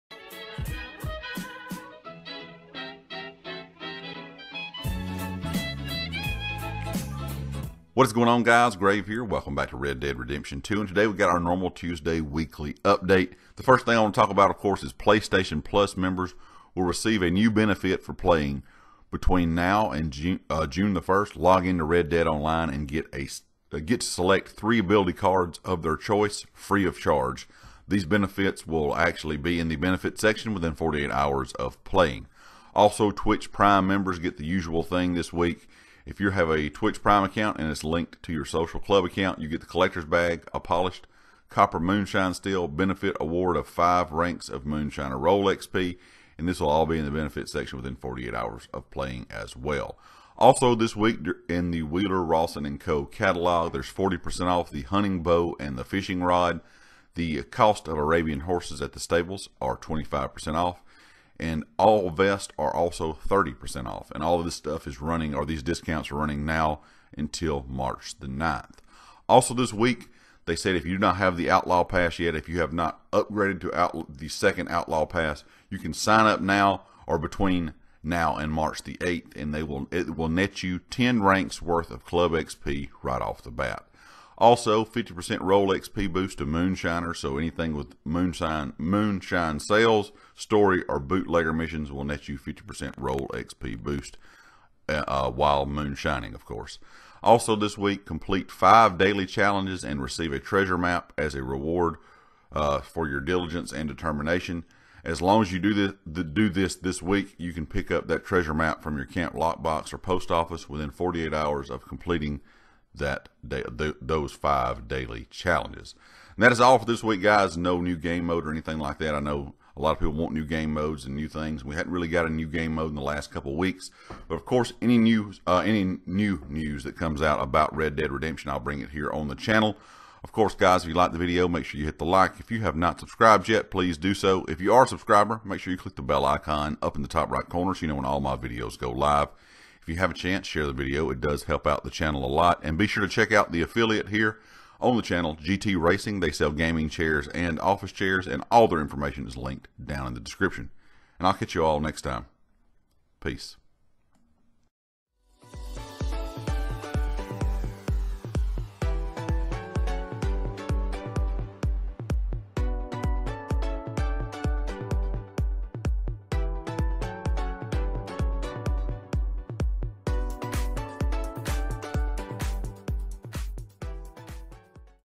What's New This Week? Red Dead Redemption 2
What's new this week in red dead redemption 2? Red Dead online weekly news wire break down. Thank's for watching!